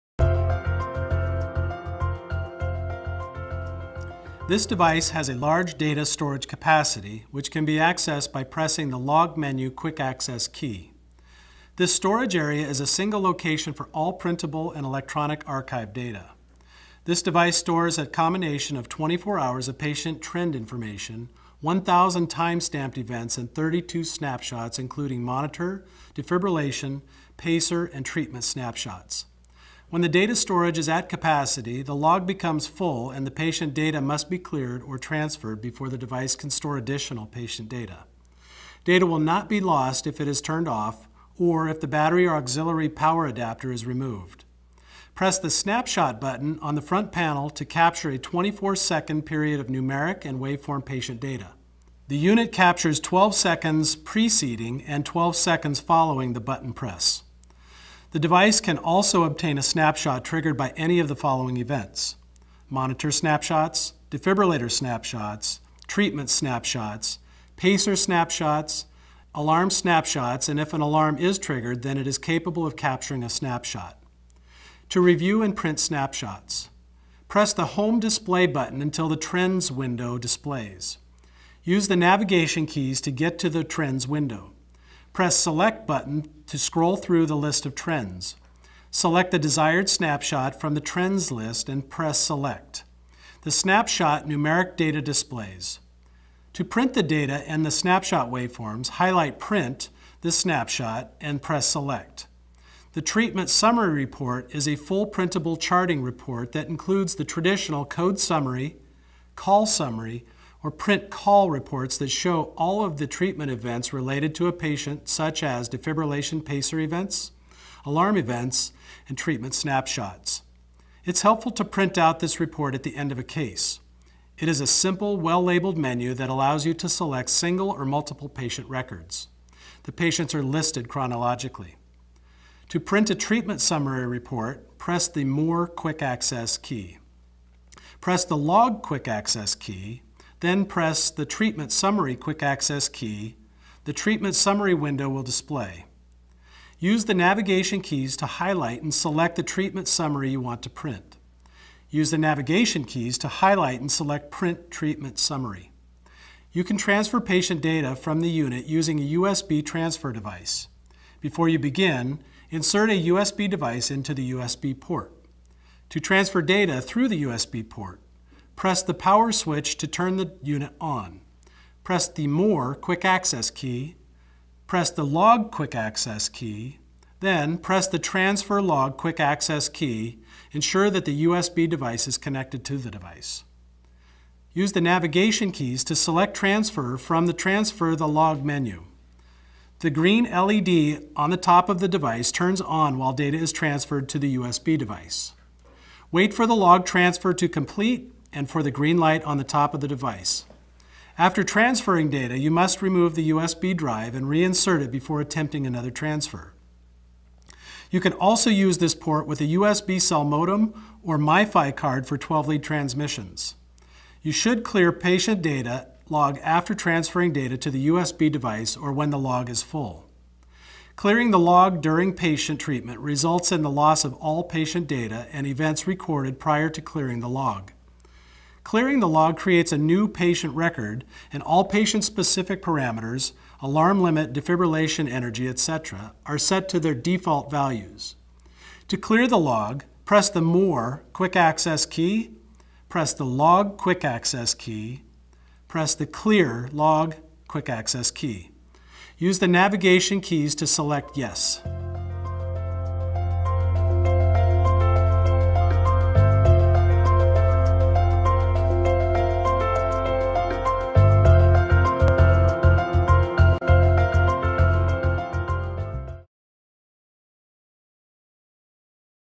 Defibrilator Zoll X Series: Comunicare și date
Defibrilatorul Zoll X Series are o mare capacitate de stocare a datelor ce poate fi accesată prin apăsarea tastei de acces rapid „Jurnal”.

Toate datele printabile și electronice se află într-un singur loc, astfel că accesarea și utilizarea lor se poate face extrem de facil.